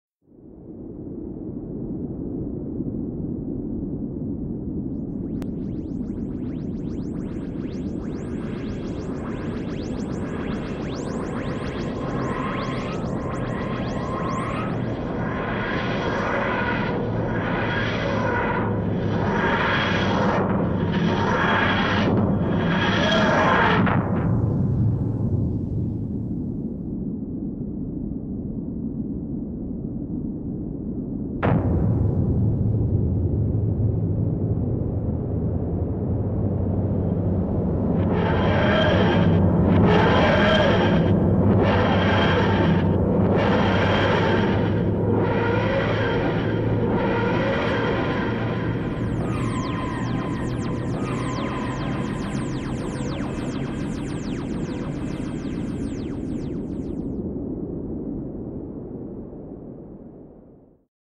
TARDIS landing in sand storm
My Attempt at the TARDIS landing in a Sandstorm in Mineimator
All models are custom and the terrain is from the program

No you may NOT use my TARDIS model and a download will NOT be released.